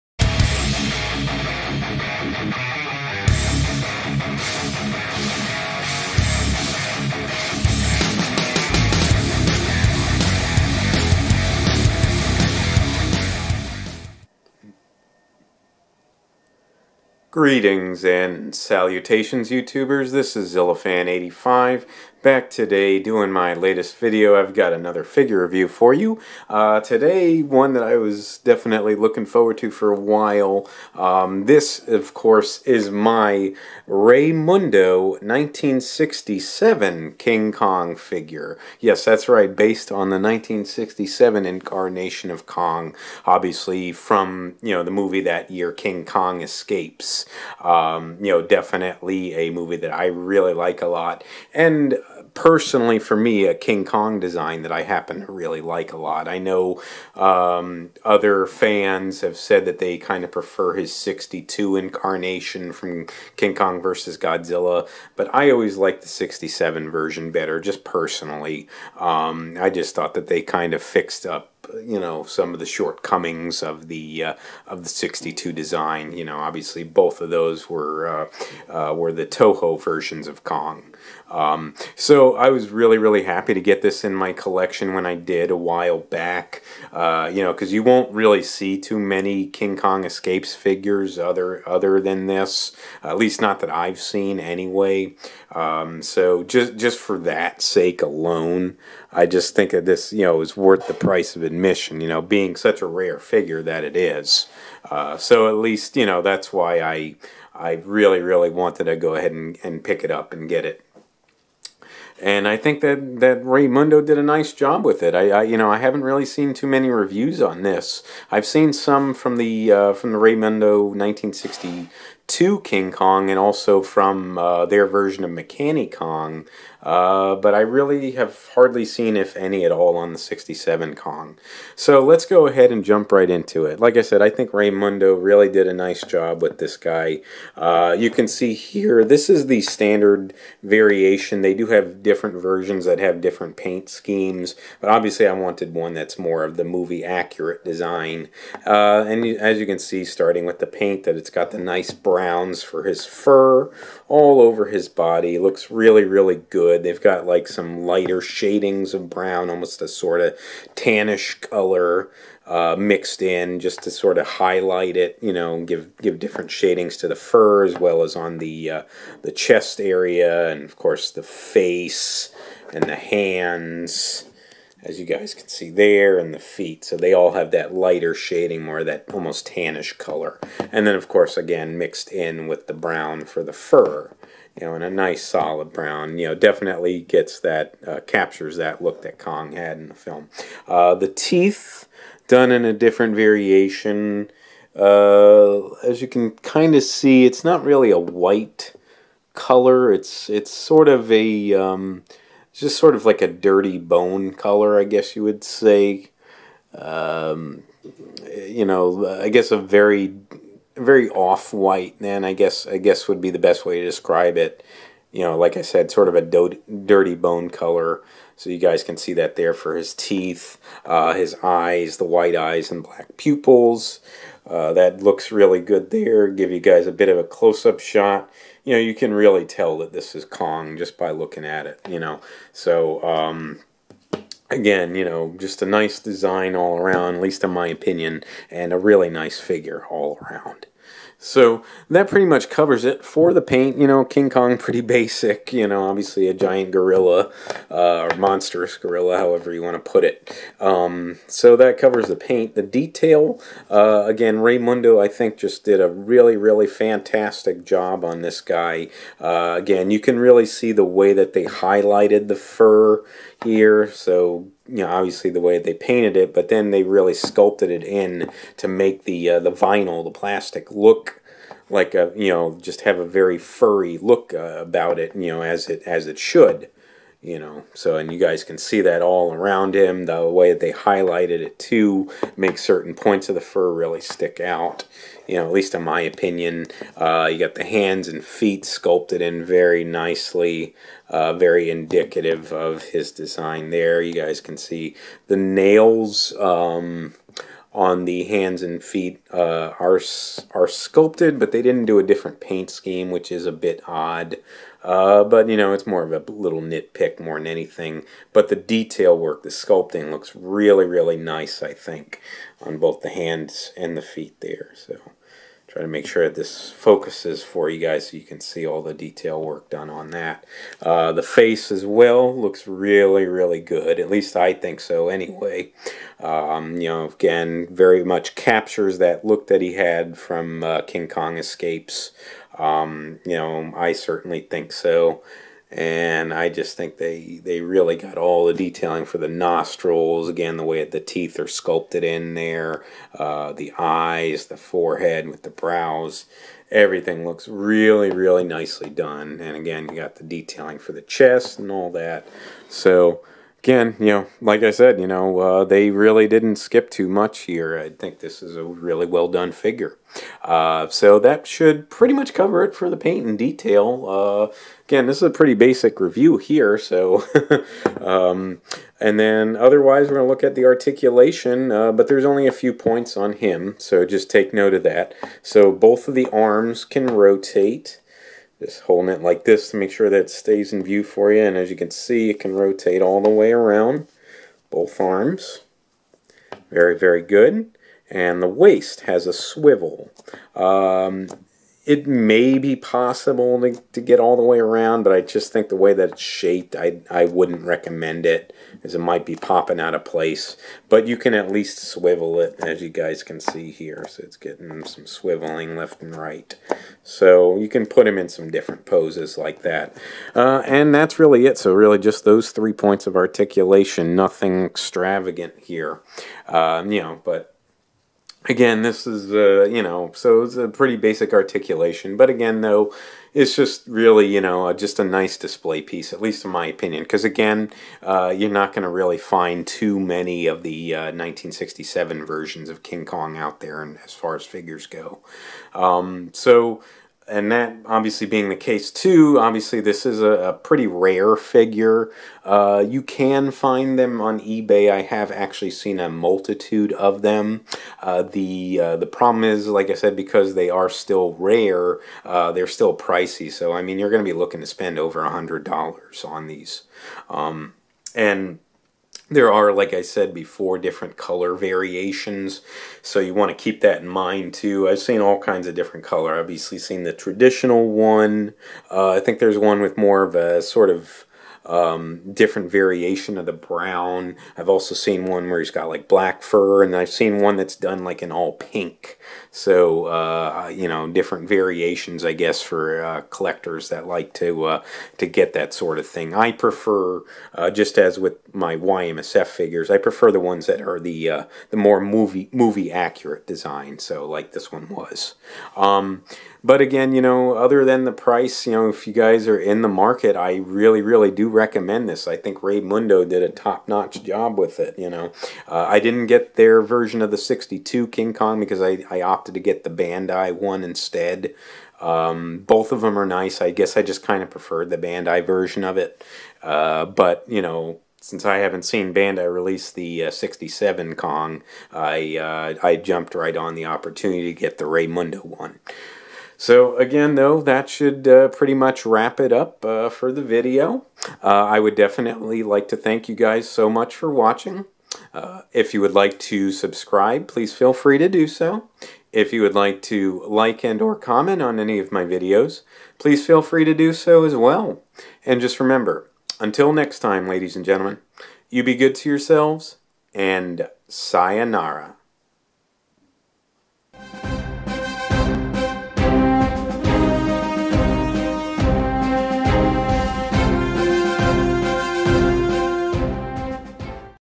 Raymundo 1967 King Kong Figure Review
This is my review of my Raymundo 1967 King Kong figure.  This figure is based on Kong's design from the 1967 film, King Kong Escapes.

I really like this version of Kong, and as I haven't really seen any other companies release it in figure form, the figure is a rare treat to get a hold of.  I think Raymundo did a great job with this figure.